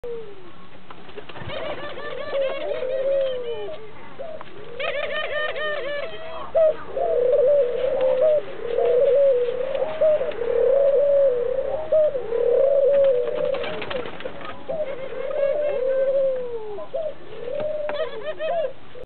Calopsitte et tourterelle
Monsieur Houpette fait un câlin à une tourterelle !!